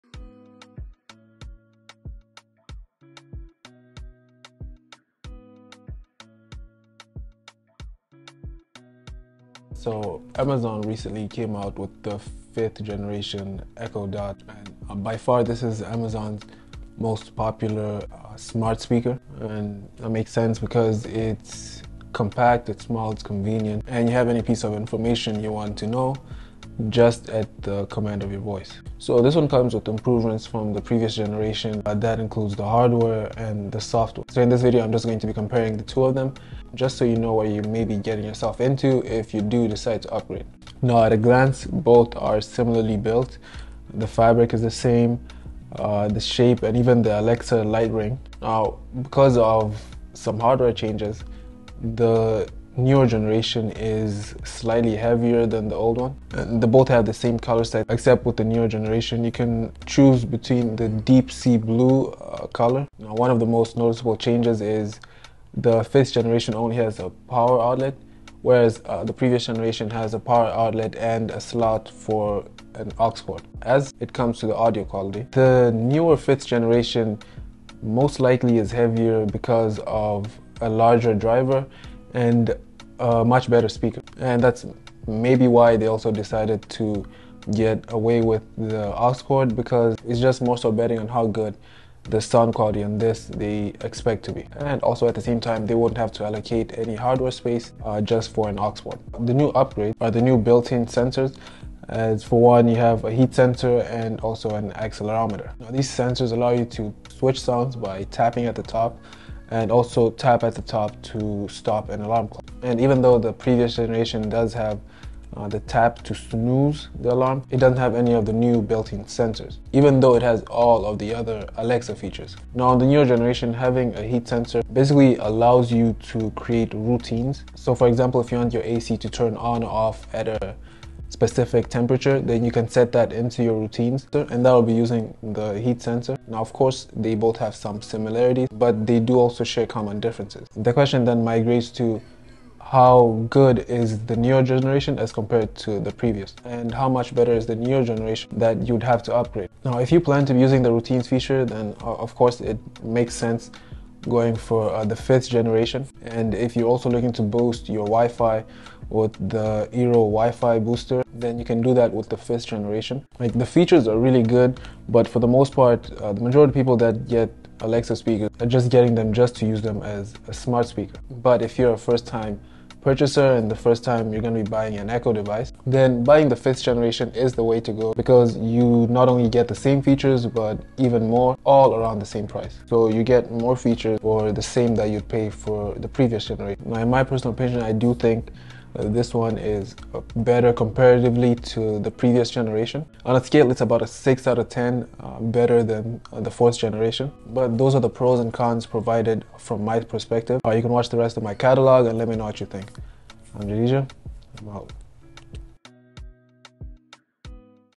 ECHO DOT 5TH GENERATION VS 4TH GENERATION
As usual Amazon has come out with their new generation echo speaker.  Hardware upgrades were the forte  for this leap into their next generation speaker.  what are you thoughts on the new gen. speaker and how did you find the video?